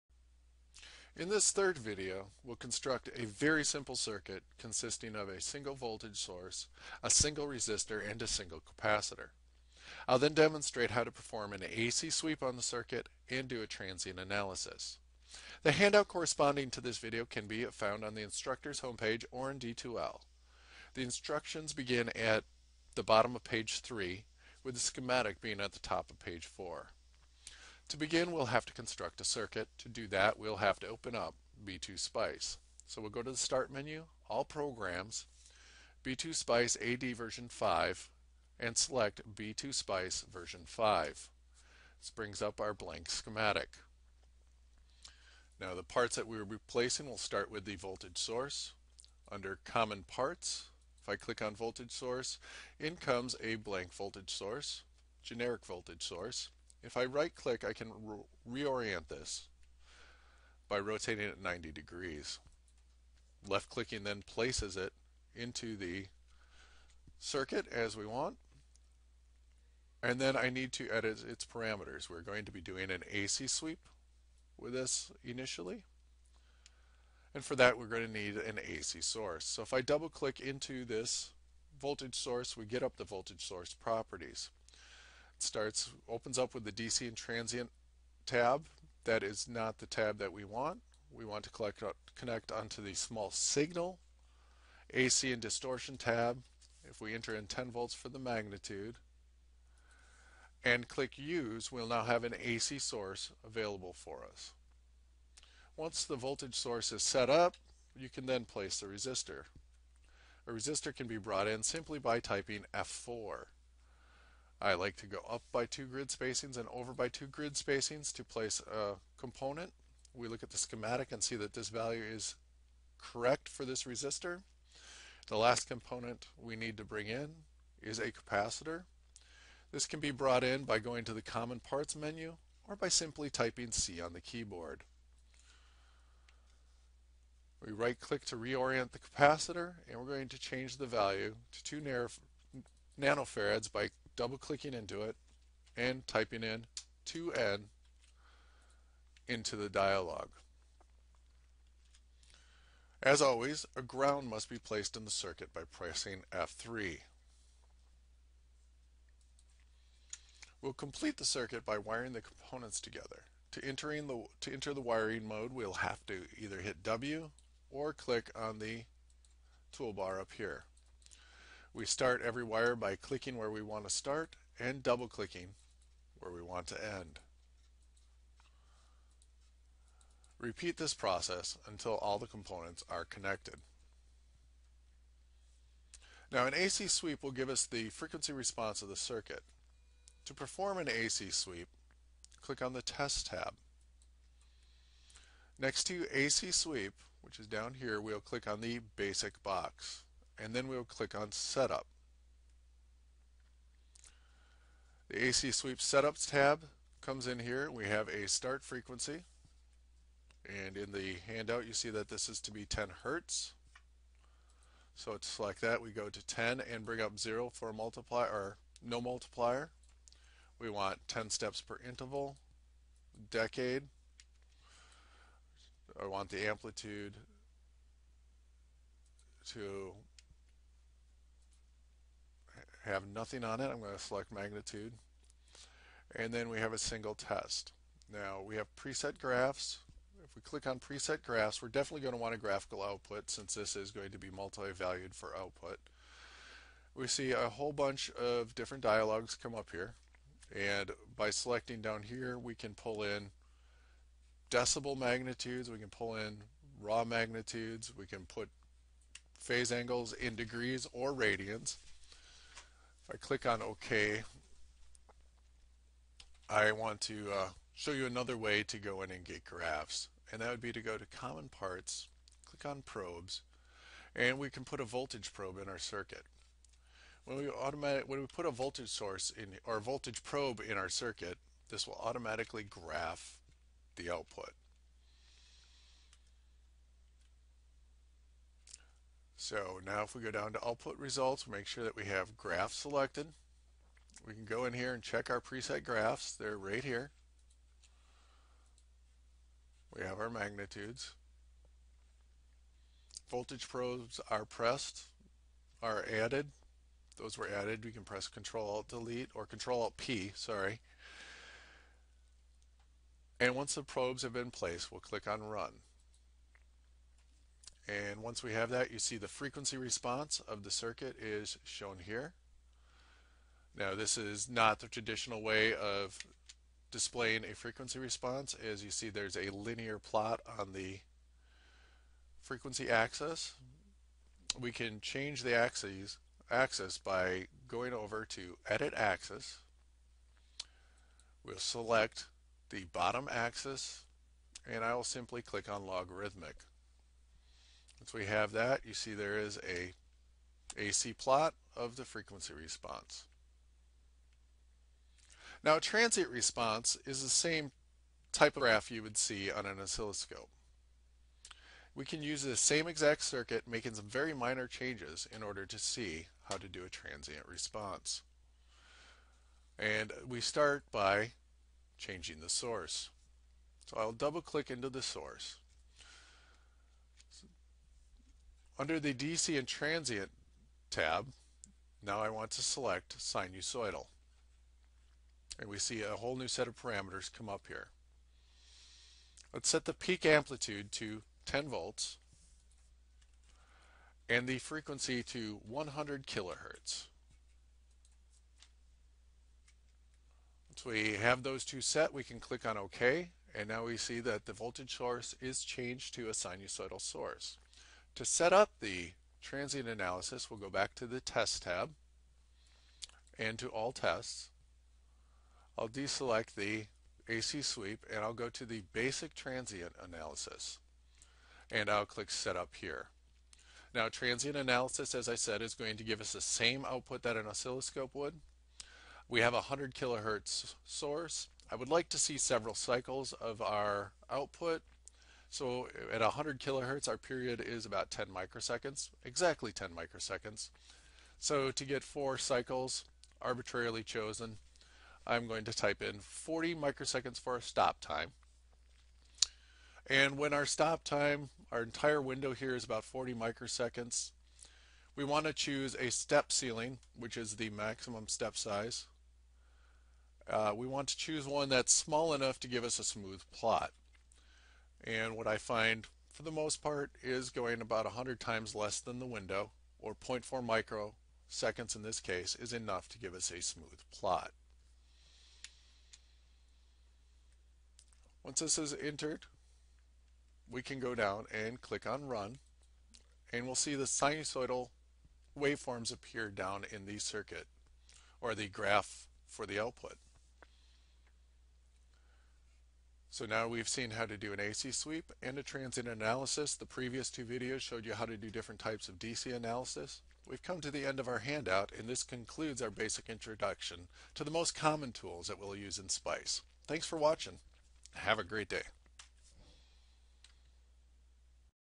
B2Spice III
A demonstration of how to do an ac sweep and a transient analysis using B2 Spice.